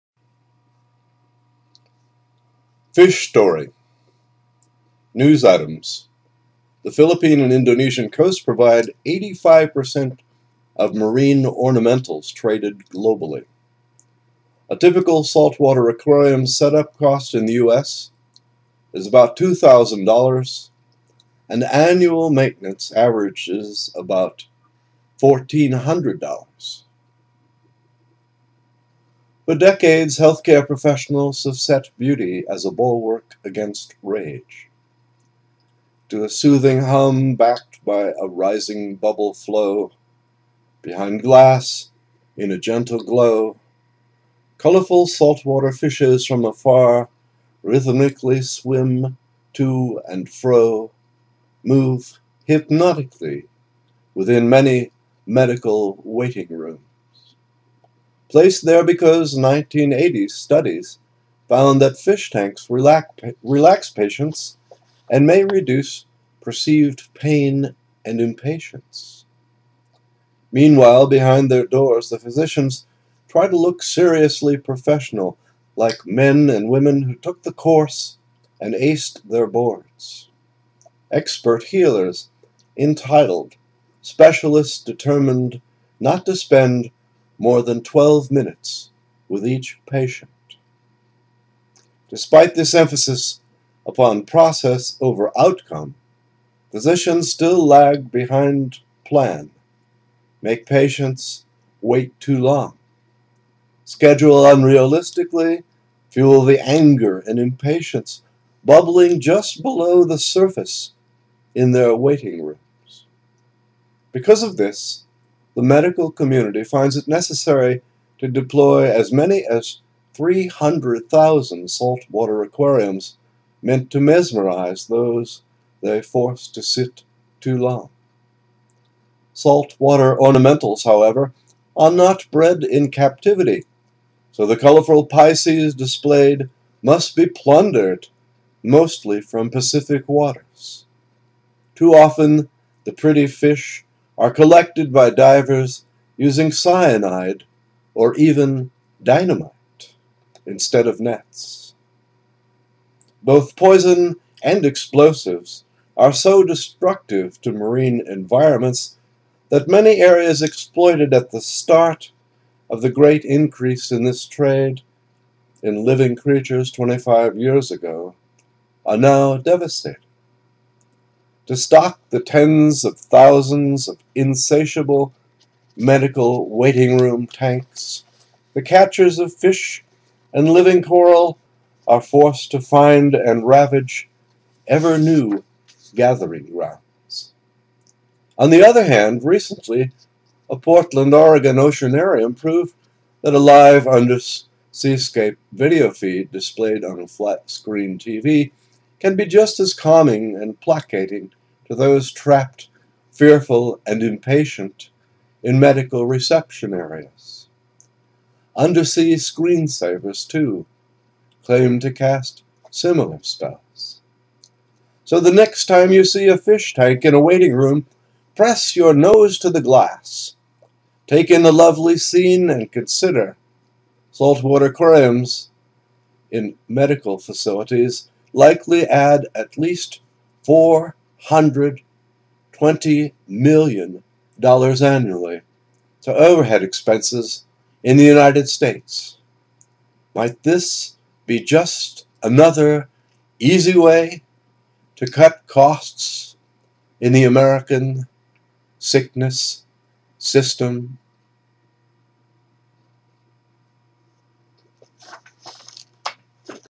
Fish Story
Do you know how much salt water aquariums may be adding to health care costs nationally?

This piece is an original work written and read by Charles Elliott/Beautyseer. It is from his chapbook, "Be Patient with Me, I am Trapped in the American Sickness System," 2010.